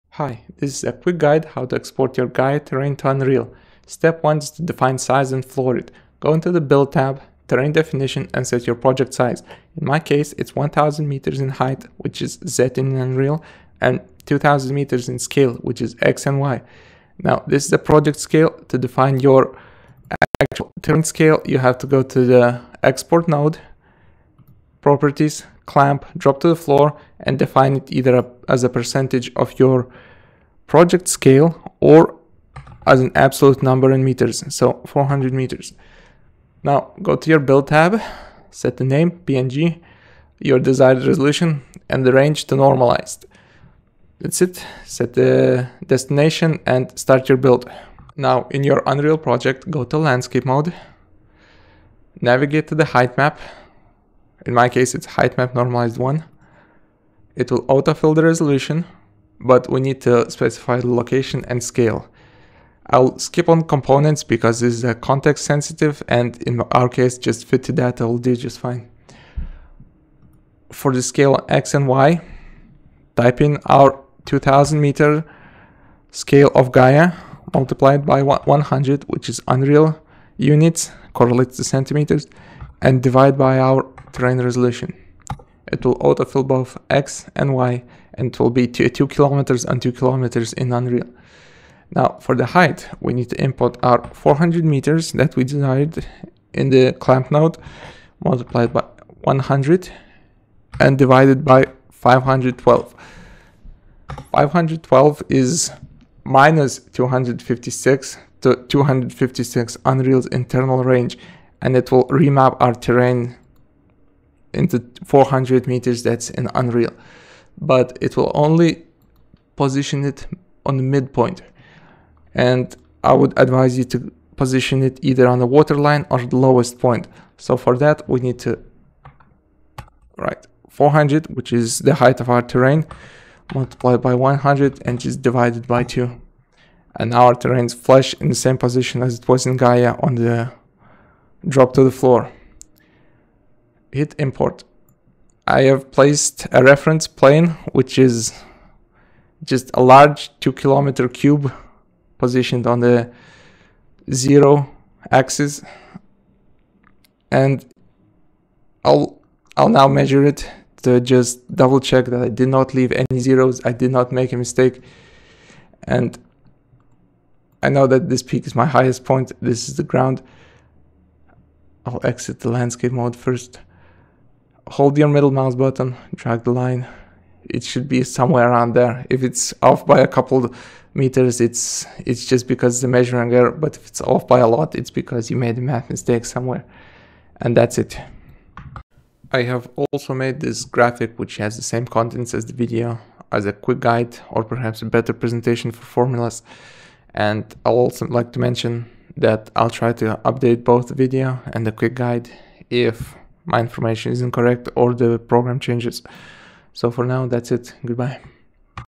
Gaea to Unreal Heightmap Import Guide
A simple guide on how to import a Gaea project heightmap to Unreal landscape. I found the existing guides to be either too long, complex, incomplete or lossy, and so I decided to make my own.

I will post a link to the graphic in comments.

Going with the normalized range is an advantage because you get the least amount of stepping, you can have enough polygons in Nanite to exceed the 16-bit range. Being able to zero-out the ground position is another thing other guides ignored.

Yeah, I know the production values are low, but I have to get this out to help others who just need a quick and complete guide on how to do a thing. I will try to do more videos on specific workflow steps in Gaea, as well as other programs I use.